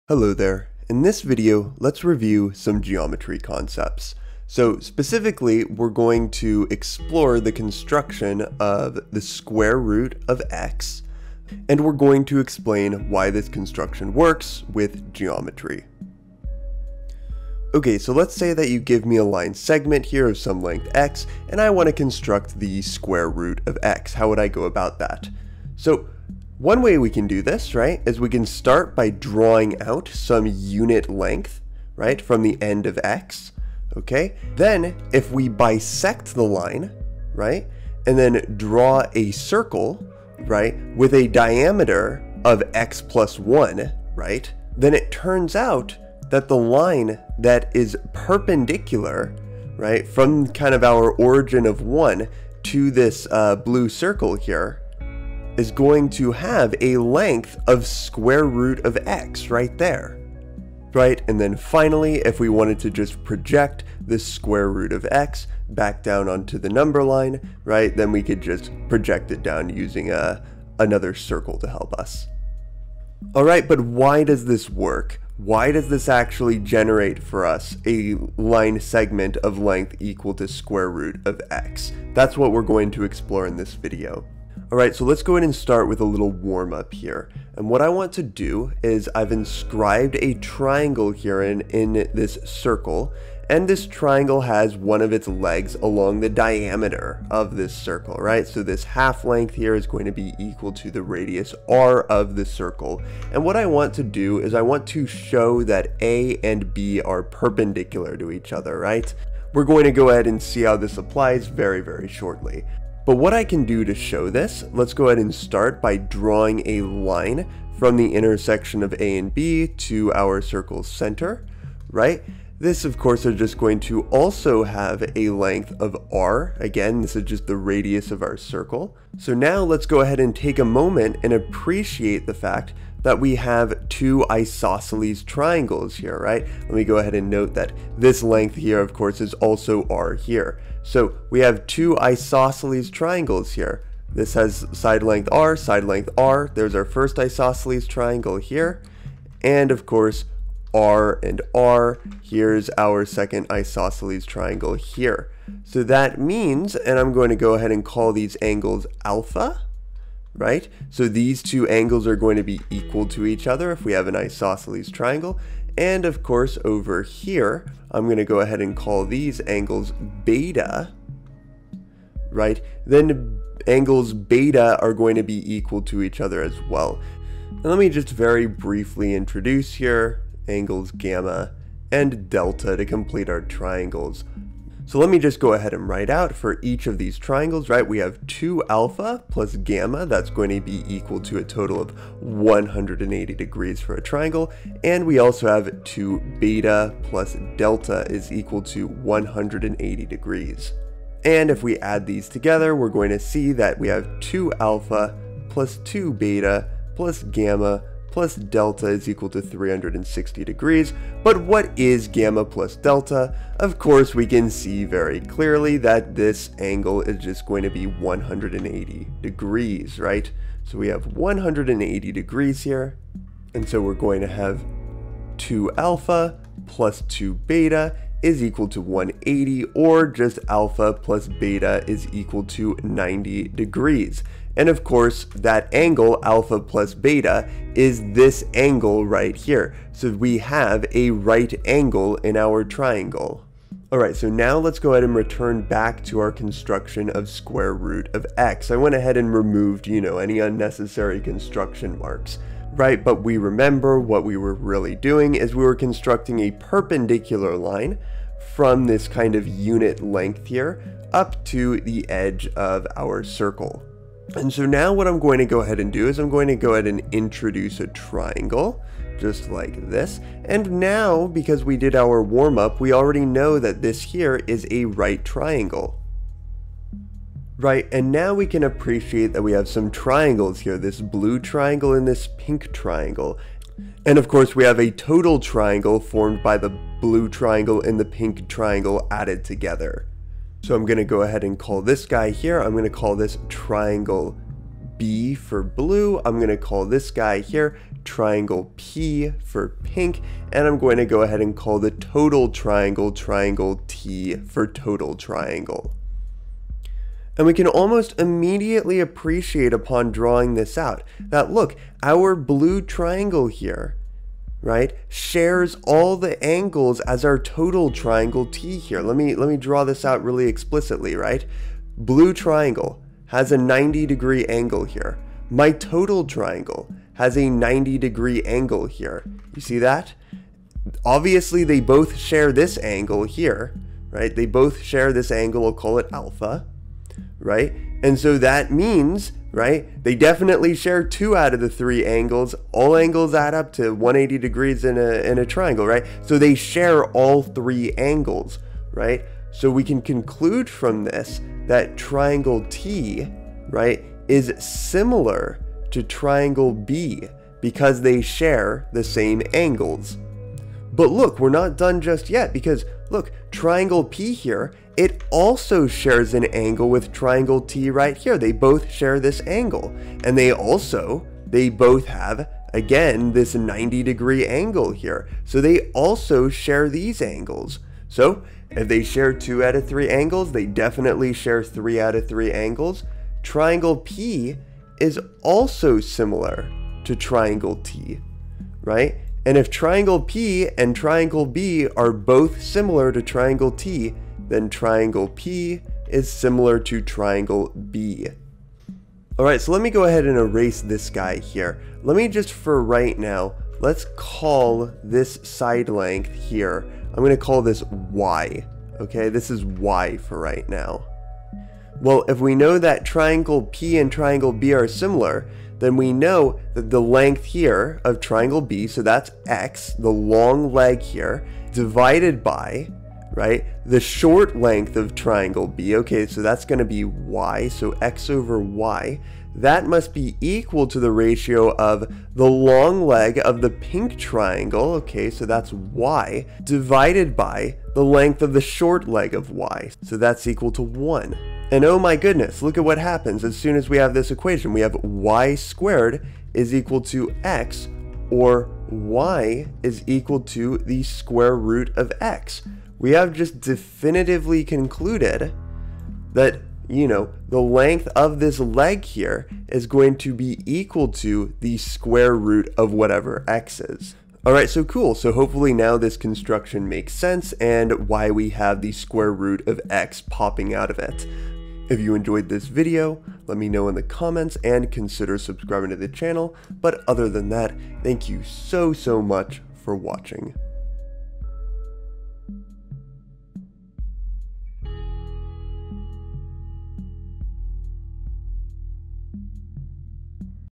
Explaining the Construction of √x | Geometry Review
In this video, we practice some geometry by explaining why a common construction method for √x works. This video was made in response to a comment question, a first for me, so that's fun! Enjoy :3